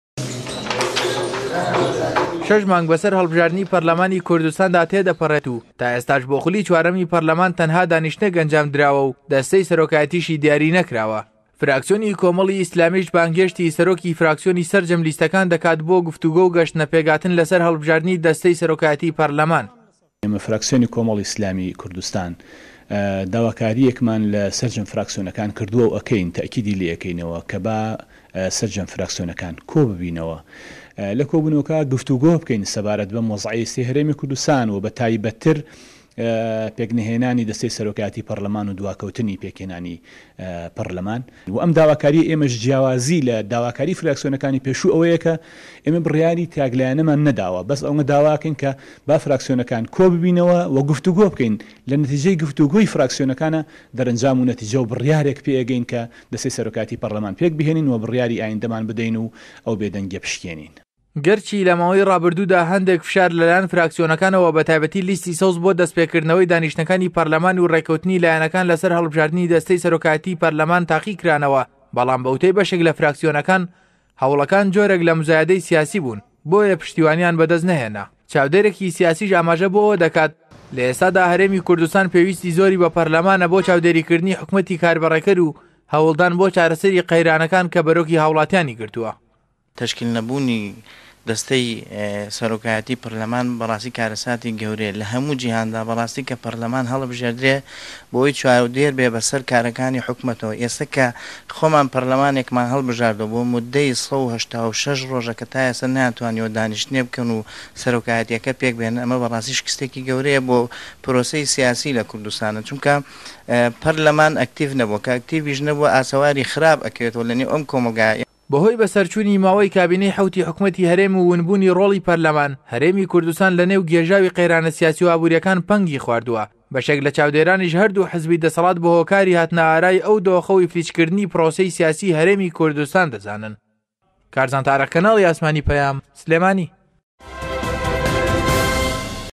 فراكسیۆنی كۆمه‌ڵی ئیسلامی داوای ئه‌نجامدانی كۆب
فراكسیۆنی كۆمه‌ڵی ئیسلامی داوای ئه‌نجامدانی كۆبونه‌وه‌ ده‌كات له‌سه‌ر دیاریكردنی ده‌سته‌ی سه‌رۆكایه‌تی په‌رله‌مان